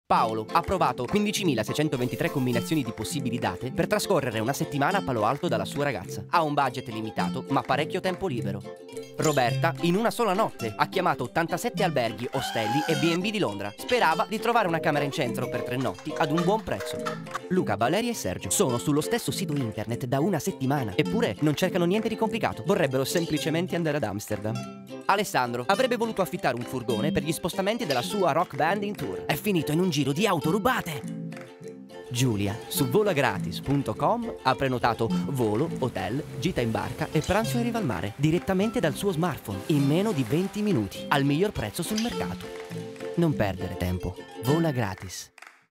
Non perdere tempo... Volagratis (web commercial)
Prenotare una vacanza dovrebbe essere un piacere, ma, a volte, può trasformarsi in un enorme spreco di tempo, se non in un vero e proprio incubo.
Non perdere tempo… Volagratis.

Web-commercial vincitore del Secondo Premio nel video-contest “Non solo voli con Volagratis.com” indetto su Zooppa.it.
Motivazione: “Per lo story telling avvincente, divertente, e per aver raccontato di un viaggio complesso da organizzare, che non si ferma solo al volo ma anche ad altri elementi”.
Video attualmente in uso sul web.

Prodotto da SPACE DONKEYS
Scritto, Diretto e Montato da ALESSANDRO TURCHI
Dop MORGAN MAUGERI
Art Director MONIA ALFIERI
Direttore di Produzione VINCENZO MARCHELLO
Storyboard e Jolly MARTINA CAMANO
Scenografia VALERIA CARIGLIA
Trucco e Parrucco VALERIA ALFIERI
Suono DAVID CUPPARI

Con
Paolo (il nerd) GIOVANNI MARIA CURRO’
Roberta (la problematica) STEFANIA PECORA
Luca, Valeria e Sergio (i fattoni) GABRIELE NUNNARI, VALERIA CARIGLIA e DARIO NACCARI
Alessandro (il musicista) VINCENZO MARCHELLO
Giulia (la bonazza) GIULIA SARA ARCOVITO
Voce LUCA STELLA

Ringraziamo LELIO NACCARI e STANI CACIA e ci scusiamo per aver dovuto tagliare le loro scene

Ringraziamo inoltre
I/O STUDIO RECORDING di Messina
LAURA LONGO e DAVID ARNAO
MIRELLA ALGERI